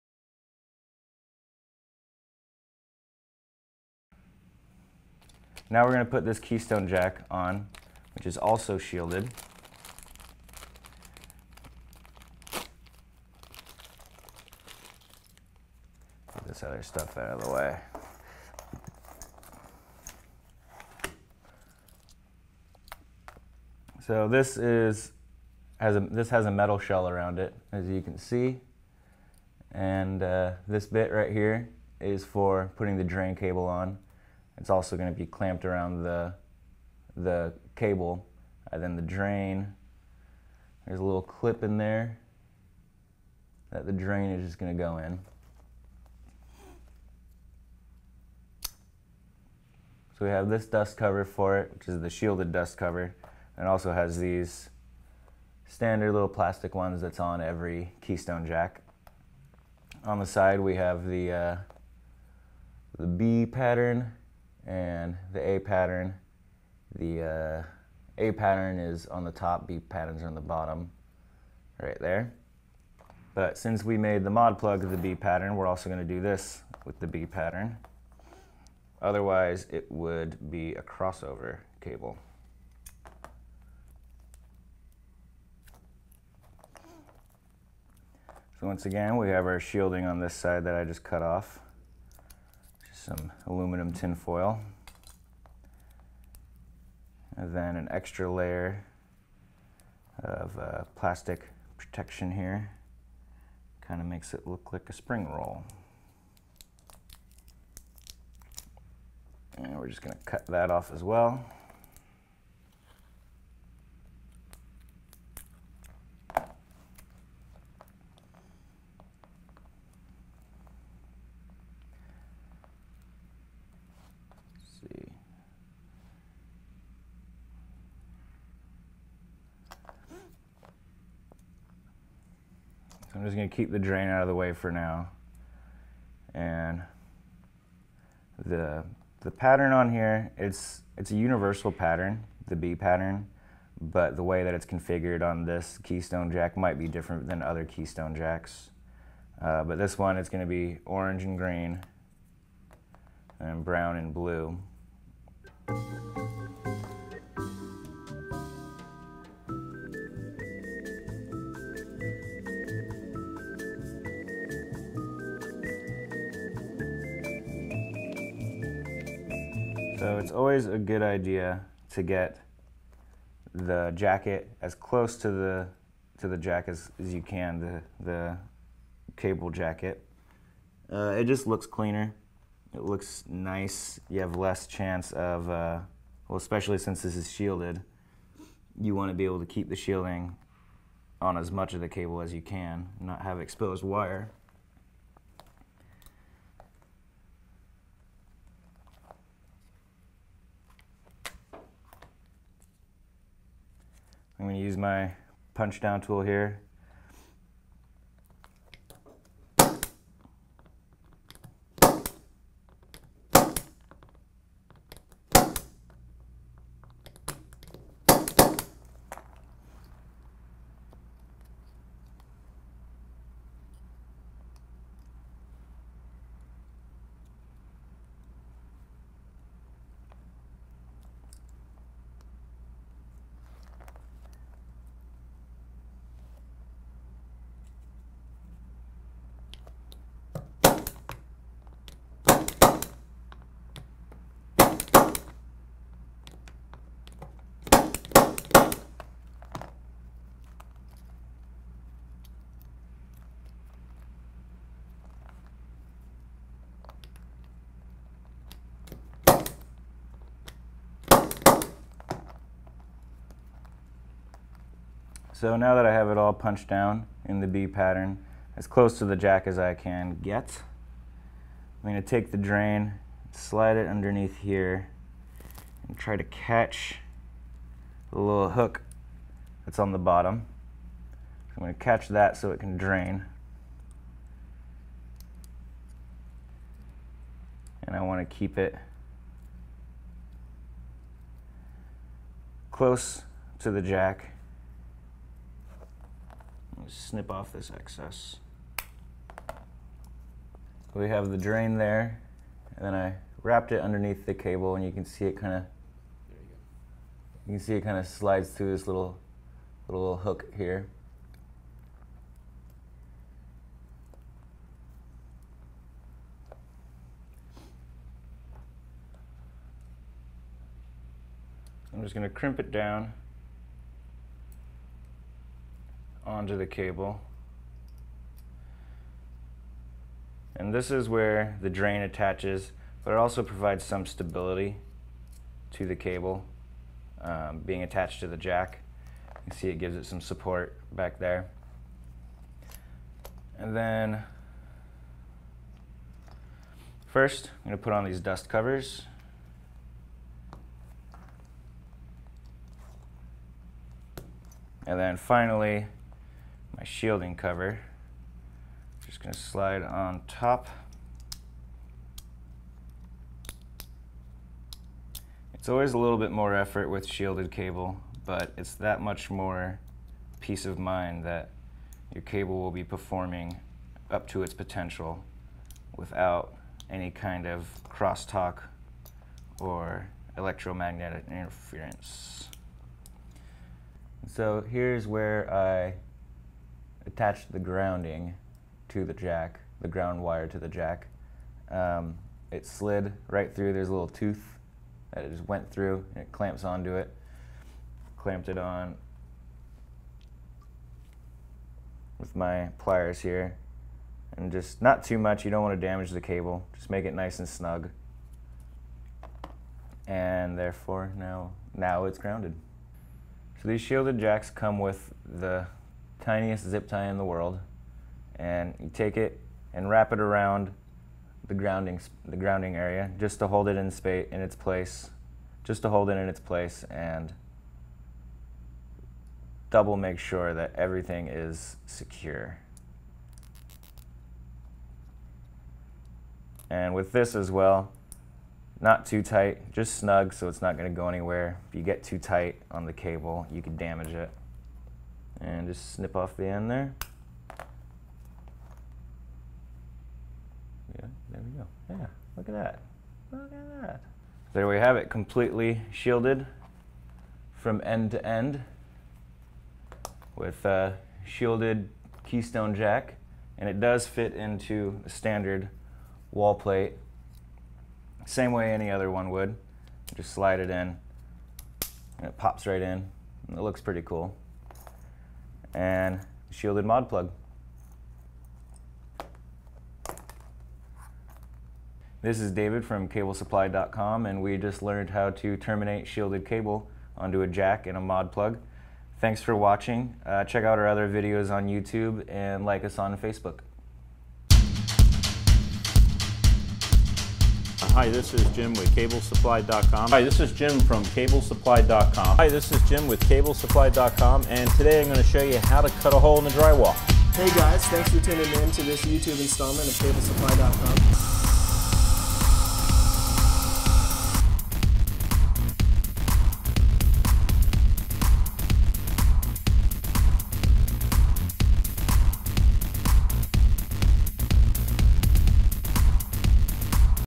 Terminating Shielded Cat6 Part 2
David Gibson shares how to terminate shielded Cat6 RJ45 Keystone Jack onto Shielded Cat6 cable.

Cat6 jacks #104069
Cat5E Jacks #104068

Products Featured:

Cat6, Cat6A Shielded Computer Keystone Jacks
Reference: 102967

SurePunch Pro Punchdown Tool from Greenlee PA3587
Reference: 102041

Shielded Cat6 Cable, 1000ft Bulk
Reference: 101189